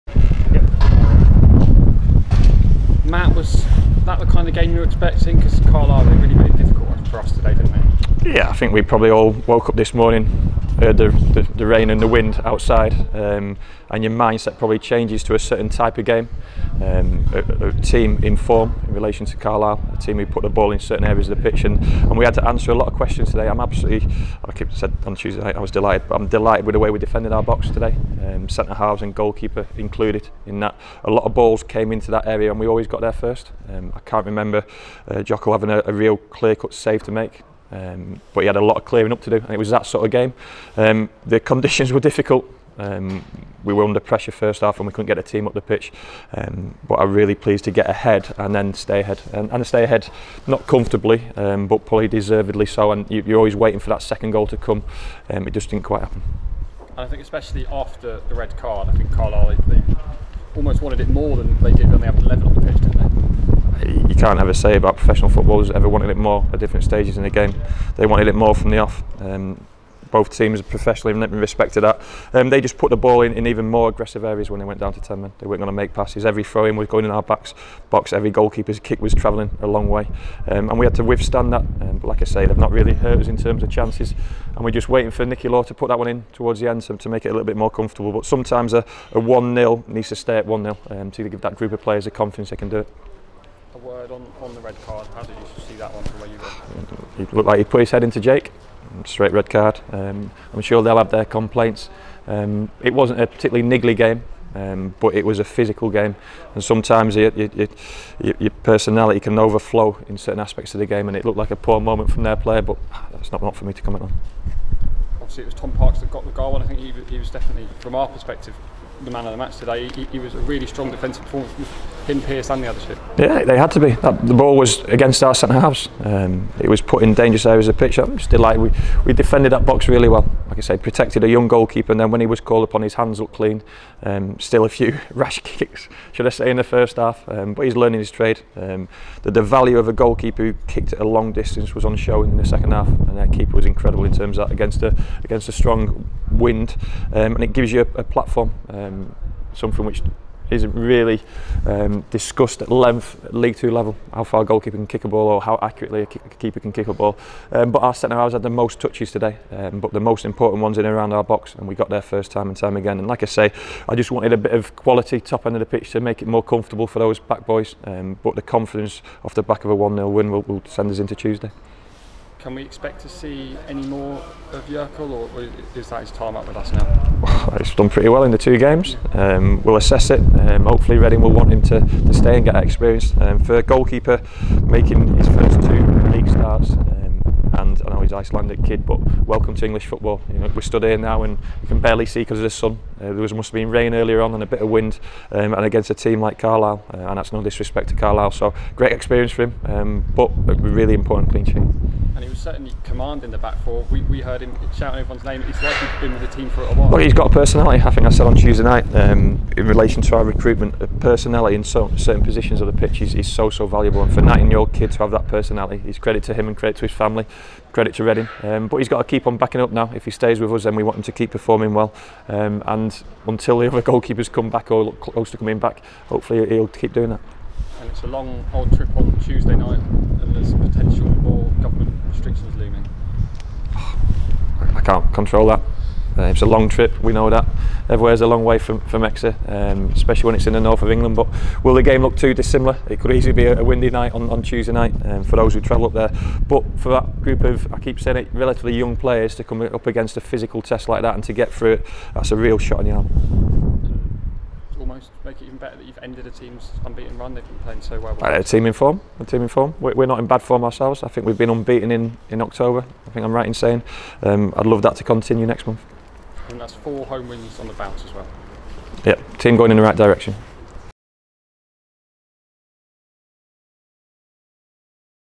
💬 Matt Taylor post Carlisle United (H) | Exeter City Football Club
💬 "We're a team heading in the right direction."

🎙 Matt Taylor reacts as City make it an unbeaten October with a 1️⃣-0️⃣ win over Carlisle United